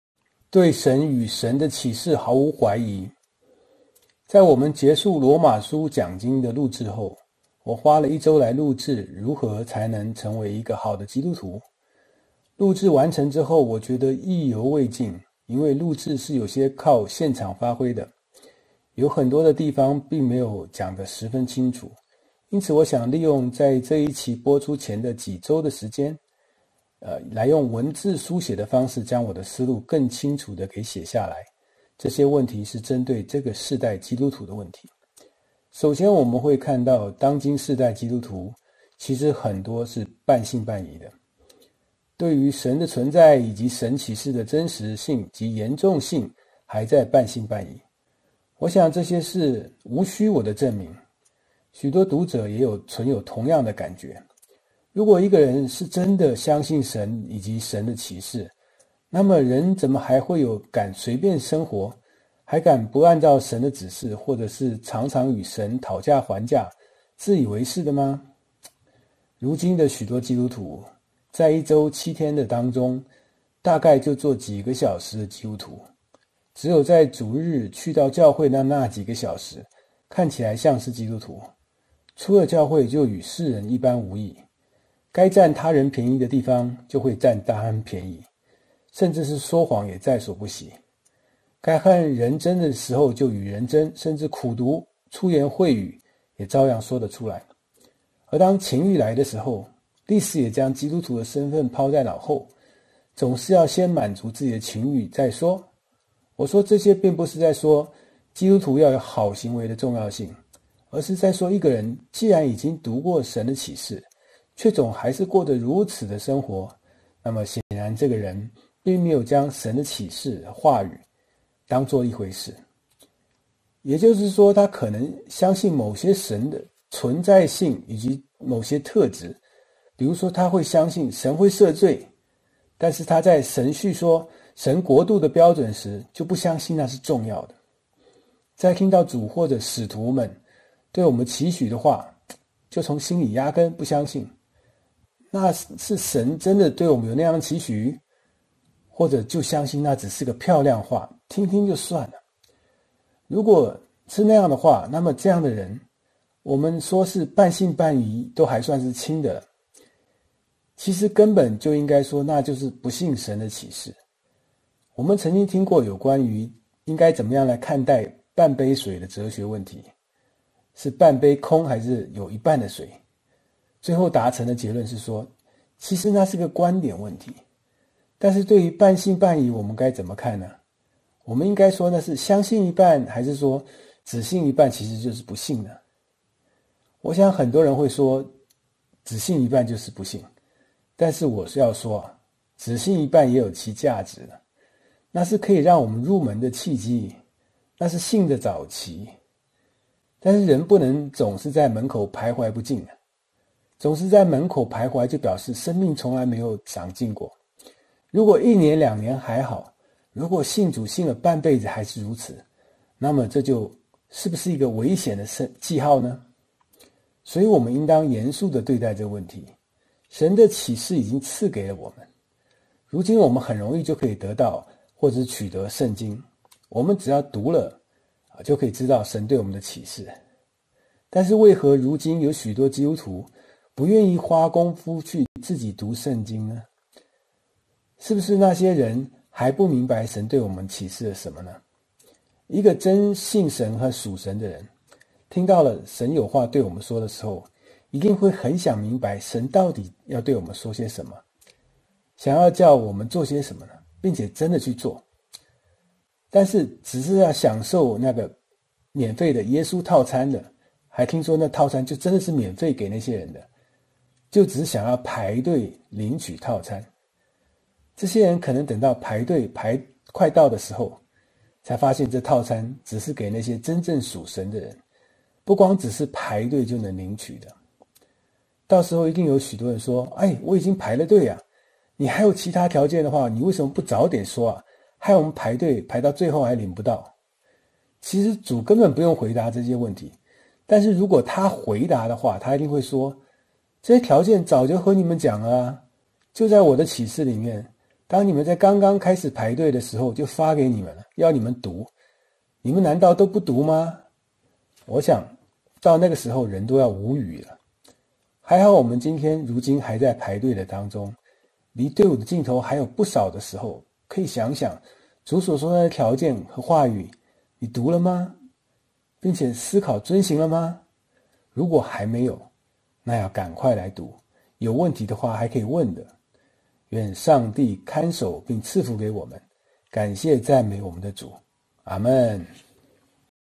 對神與神的啟示毫無懷疑
在我們結束羅馬書講經的錄製後，我花了一週來錄製了如何才能成為一個好的基督徒；錄製完成之後我覺得意猶未盡，因為錄製是有些在靠現場發揮，有很多的地方並沒有講得十分清楚，因此我想利用在這一期播出前的幾週時間，來用文字書寫的方式來將我的思路更清楚的給寫下來，這些問題是針對這個世代基督徒的問題。

首先我們看到當今世代的基督徒有很多其實是半信半疑的！對於神的存在，以及神啟示的真實性及嚴重性還在半信半疑。我想這些事無需我來證明，很多讀者們也都存有同樣的感覺。如果一個人是真的相信神與神的啟示，那麼人怎麼還有敢隨便的生活嗎？還敢不按照神的指示，或者是常常與神討價還價還自以為是嗎？

如今的許多基督徒，在一週七天的當中大概就做幾個小時的基督徒，只有在主日去到教會的那幾個小時看起來像是個基督徒，出了教會就與世人一般無異，該佔他人便宜的地方就會佔人偏宜、甚至說謊也再所不惜；該和人爭就與人爭，甚至苦毒、粗言穢語也照樣說出來。

而當情慾來的時候，立時也將基督徒的身份拋在腦後了，總是要先滿足自己的情慾再說；我說這些並不是在說基督徒要有好行為的重要性，而是在說一個人既然已經讀過神的啟示，卻還總是過著如此的生活，那麼顯然這個人並沒有將神所啟示的話語當作一回事。

也就是說他可能相信某些神的存在與某些特質，比如說他相信神會赦罪，但是他在 神述說神國的標準時，就不相信那些是重要的，在聽到主或者是使徒們對我們的期許的話，就從心裡壓根不相信、那是神真的對我們有那樣的期許，或者就相信那只是個漂亮話，聽聽就算了；

如果是那樣的話，那麼這樣的人、我們說是半信半疑都還算是輕的了，其實根本就應該說那就是不信神的啟示。我們曾經聽過關於要怎樣看待半杯水的哲學討論，最後達成的結論說、其實那是個觀點的問題。但是對於半信半疑我們該怎麼看呢，我們應該說那是相信一半嗎？還是說只信一半其實就是不信呢？

我想很多人會說只信一半就是不信，但我要說只信一半也有其價值，那是可以讓我們入門的契機；那是信的早期。但是人不能總在門口徘徊不進，總是在門口排徊就表示生命從來沒有長進。如果一年兩年還好，如果信主了半輩子還是如此，那麼這是不是就是個危險的訊號？

所以我們應當嚴肅的對待這個問題，神的啟示已經賜給了我們；如今我們很容易的就可以得到或是取得聖經，我們只要讀了就可以知道神對我們的啟示，但是為何如今有許多的基督徒不願意花功夫自己去讀聖經呢？是不是那些人就是還不想明白神對我們啟示了什麼。

一個真信神和屬神的人、聽到了神有話對我們說的時候，一定會很想明白神到底想要對我們說什麼，想要叫我們做些什麼，並且真的去做。但是只是想要享受耶穌套餐的、還聽說那套餐是免費的那些人，就只想要排隊等著領取那套餐，這些人可能等到排隊排到的時候，才發現這套餐是只給那些真正屬神的人，不光只是排隊等就能領取的。

到時候一定有許多人說我已經排隊了啊，你還有其它條件的話為什麼不早點說啊，害我們排了隊最後也領不到！其實主根本不用回答這些問題，但是如果祂回答的話，祂一定會說：這些條件早就和你們講了啊，就在我的啟示裡面，當你們在剛剛開始排隊的時候就發給你們了呀、要你們讀，你們難道都不讀嗎？

我想到那個時候人都要無語了；還好我們如今還在排隊的當中，離隊伍的盡頭還有不少的時間；可以想想、主所說的那些條件和話語你讀了嗎、而且思考並遵行了嗎？如果還沒有、那要趕快來讀，有問題的話還可以問的。願上帝看守並賜福給我們，感謝讚美我們的主。